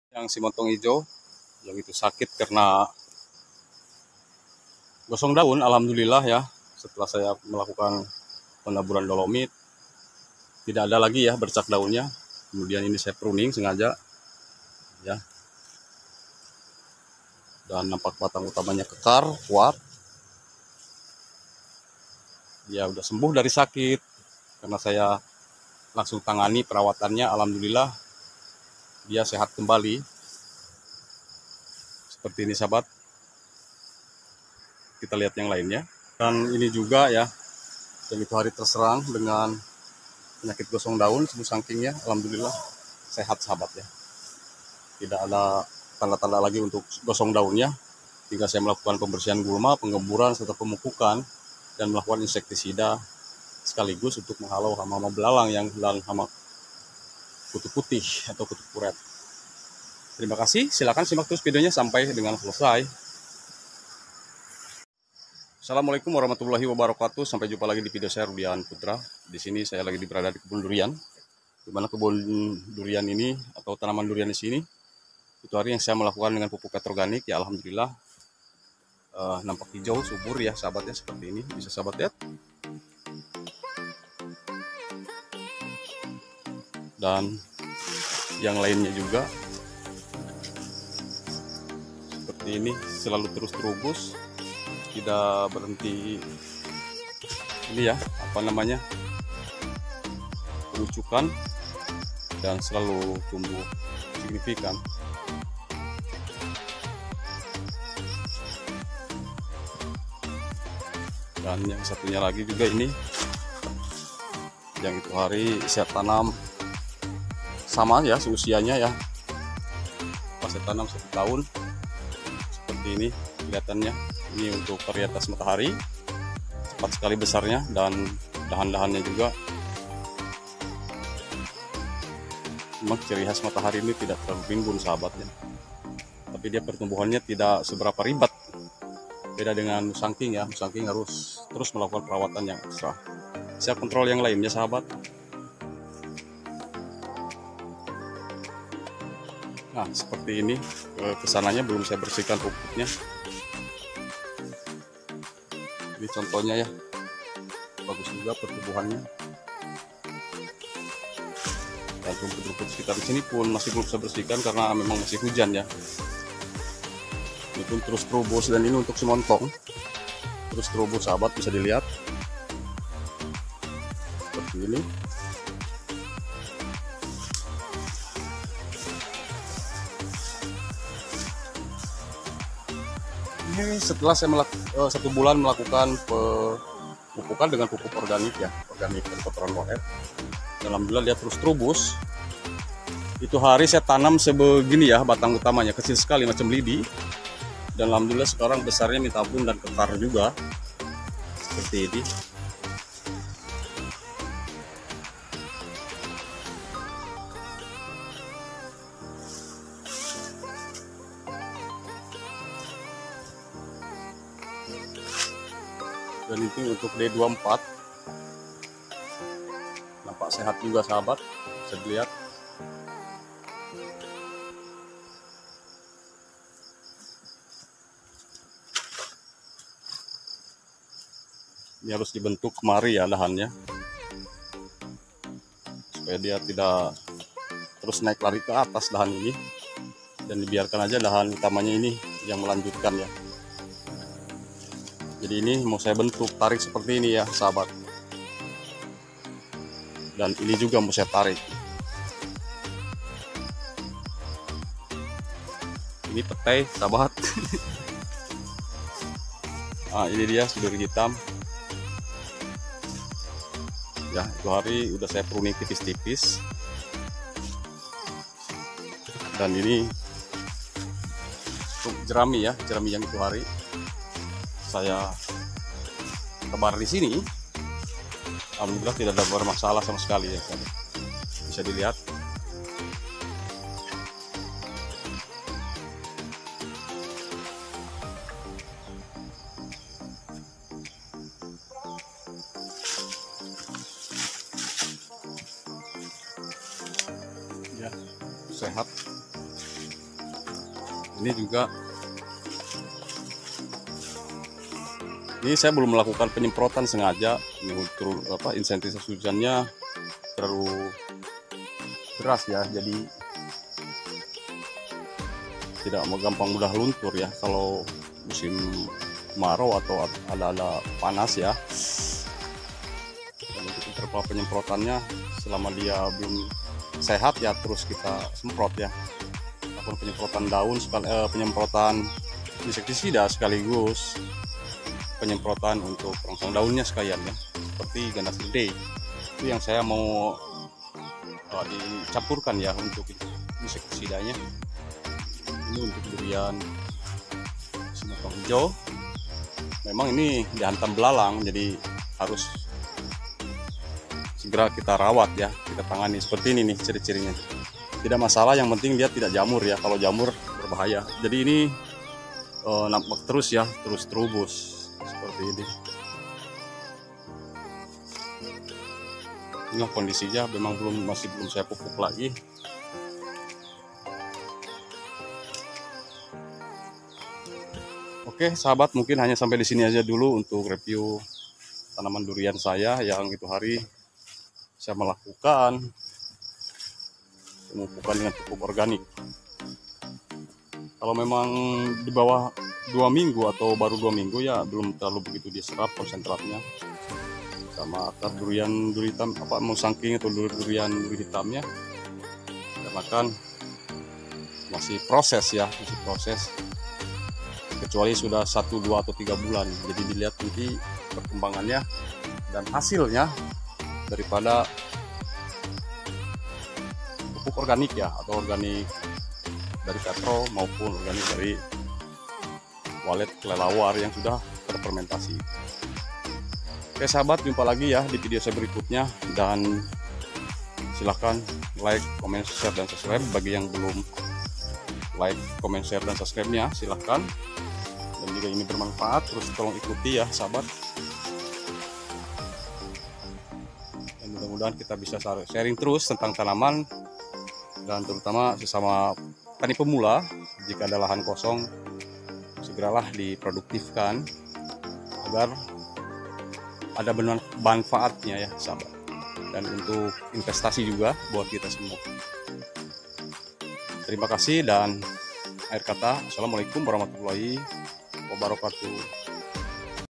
Pengontrolan Kebun Durian Di Saat Musim Hujan Secara Rutin jangan sampai Terlena Dan Terbengkalai
Hallo Sahabat Rudian putra..Sampai jumpa lagi di video kesempatan saya kali ini..di video ini saya telah melakukan pengontrolan rutin di saat musim hujan Supaya tanaman kita tetap sehat segar dan aman tidak terserang penyakit...Silahkan Simak terus video nya sampai dengan selesai dan jangan lupa tolong bantu Like komen Share dan subscribe..terimakasih 🙏🙏